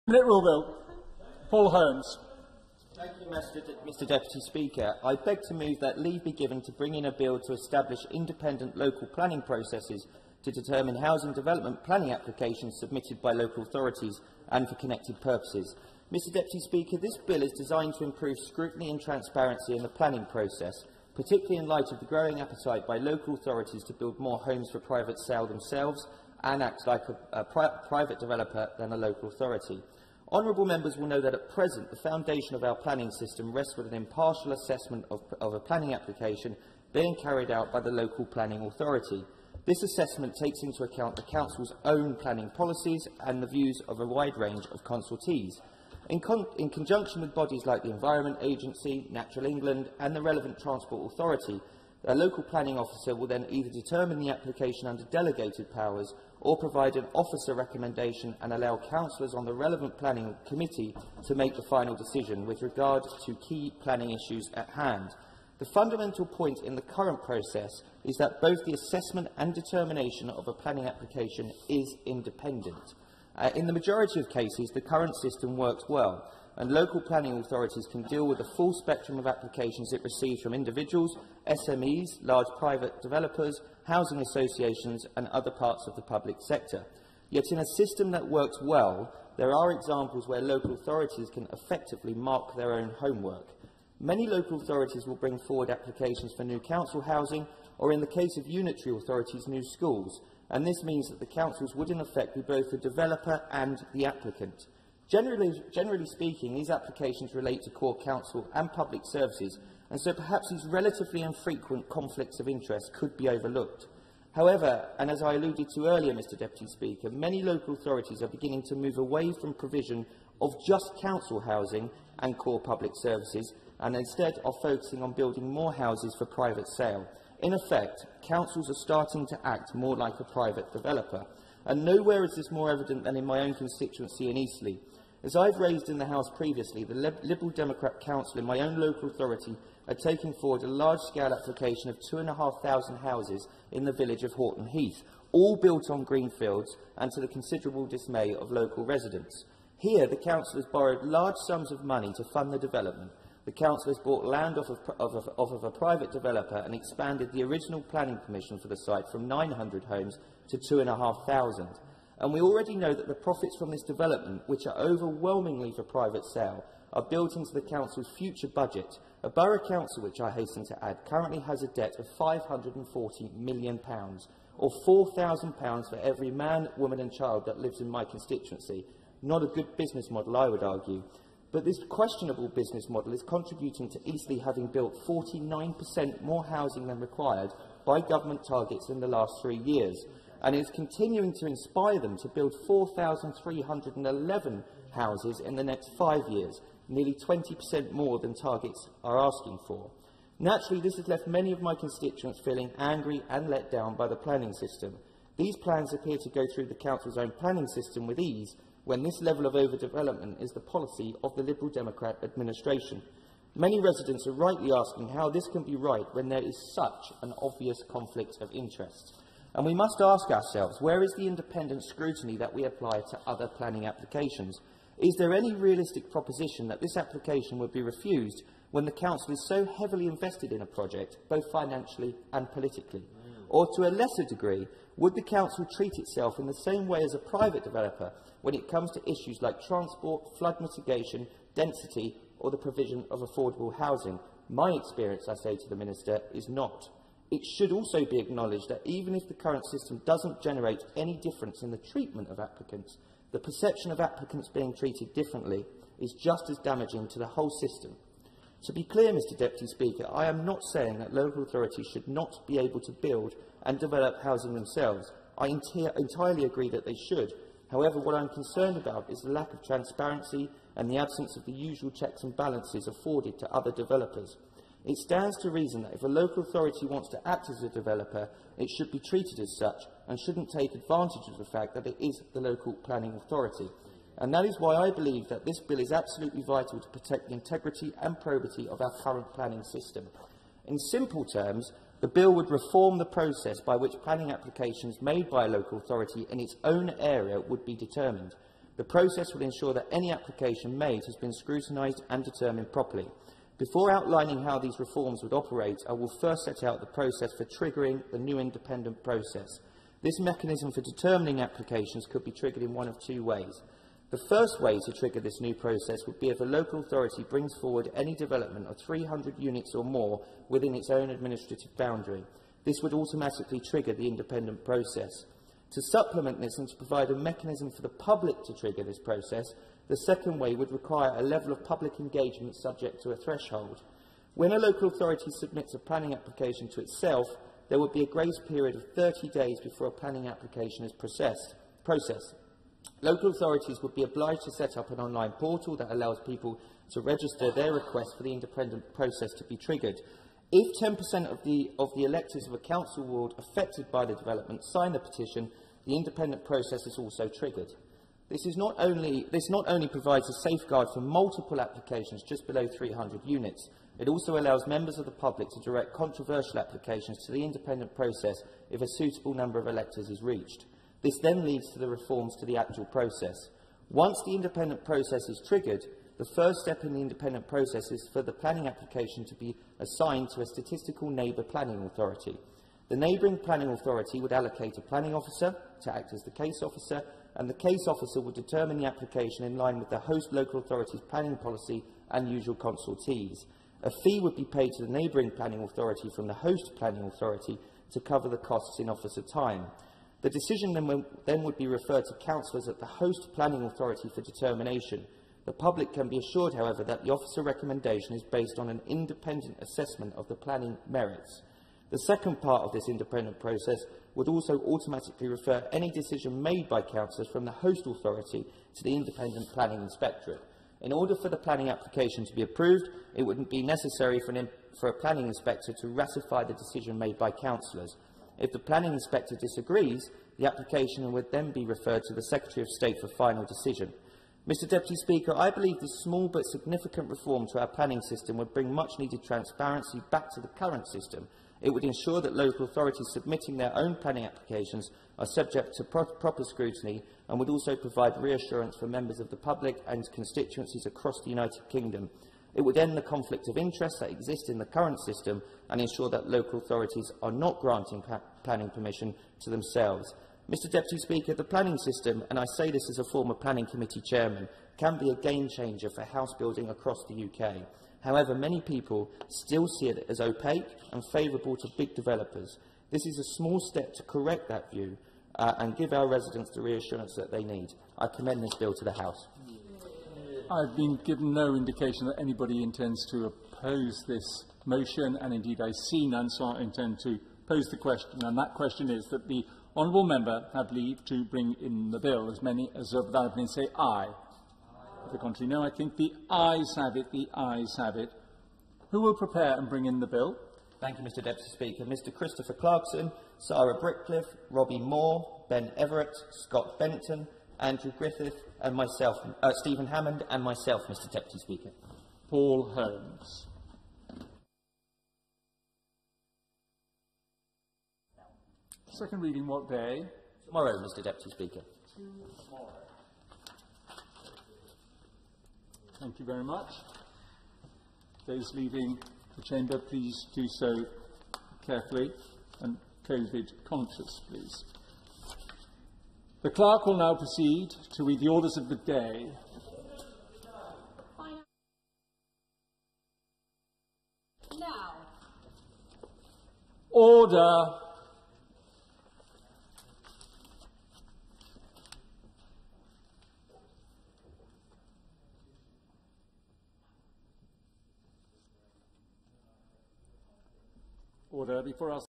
Paul Holmes - Local Authority Housing Developments
My proposed bill to stop Council's awarding themselves planning permission and acting like a private developer